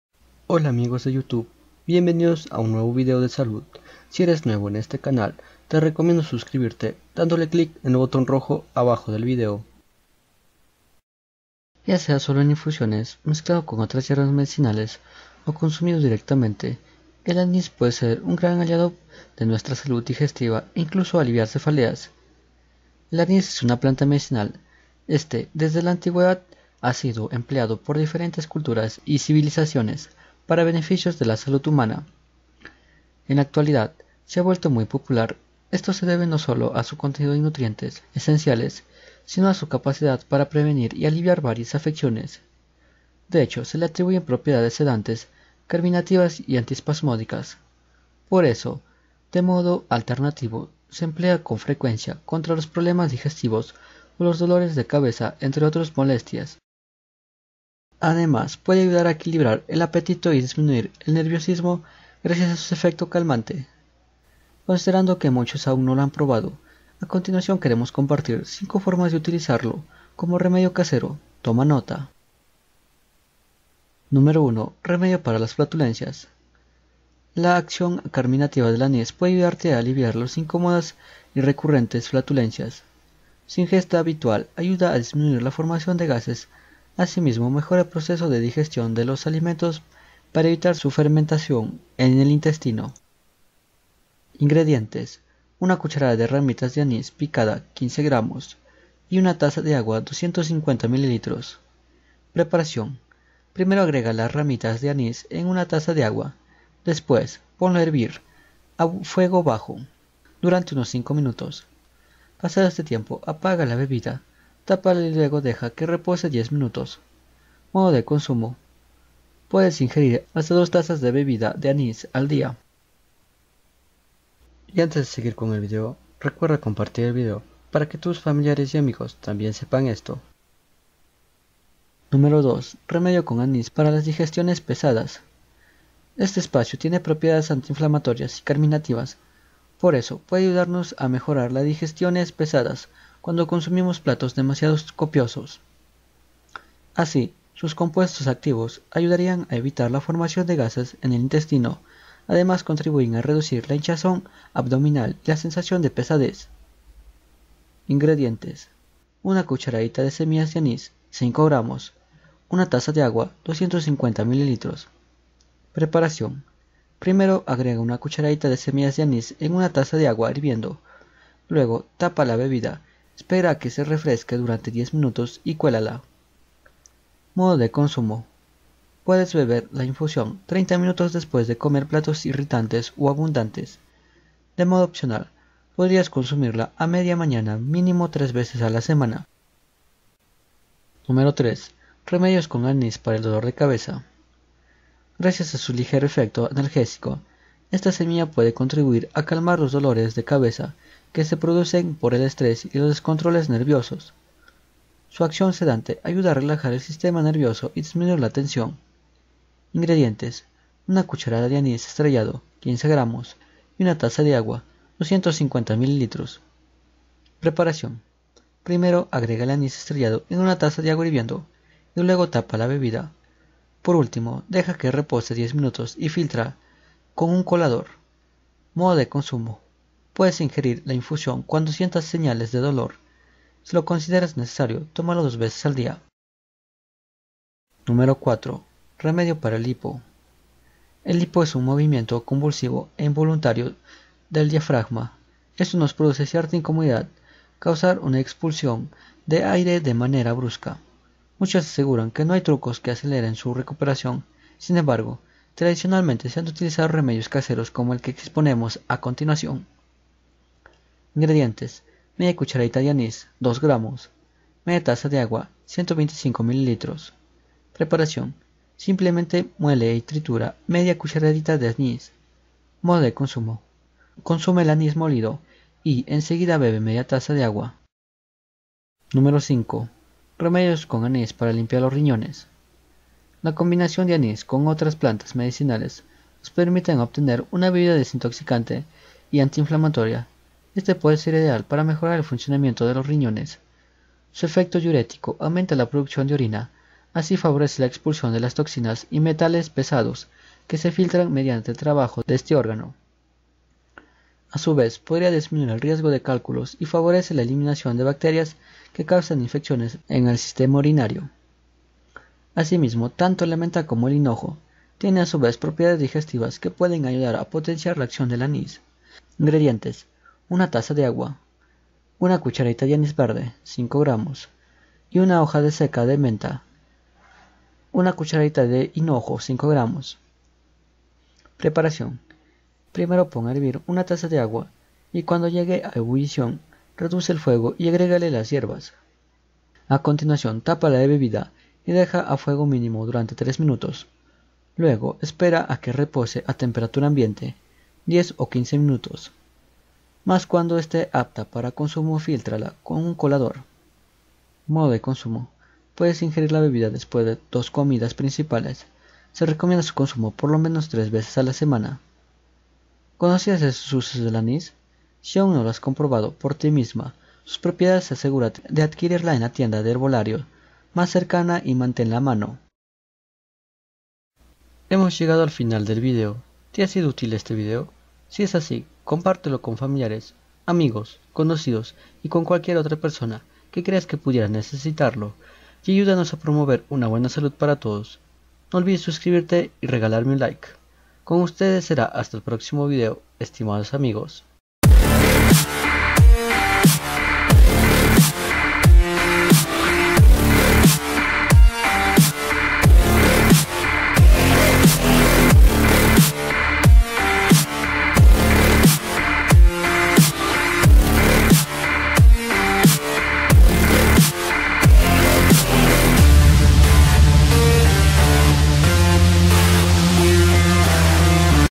BENEFICIOS Y PROPIEDADES DEL ANIS ESTRELLADO Y SUS SEMILLAS
BENEFICIOS Y PROPIEDADES DEL ANIS ESTRELLADO Y SUS SEMILLAS - el anis estrella tiene múltiples usos  lo puedes usar para aliviar dolores estomacales o digestivos  sirve como un excelente expectorante  ayuda  a controlar el nivel de azúcar en la sangre  también mejora la irregularidad del periodo en las mujeres  sin duda las propiedades de anis estrellado son muy beneficiosas para mejorar muchos problemas de la salud de tu organismo...

propiedades y beneficios del anis ¿para qué sirve el te de anis? anis estrellado para el cabello 🐷 propiedades curativas del anis estrellado.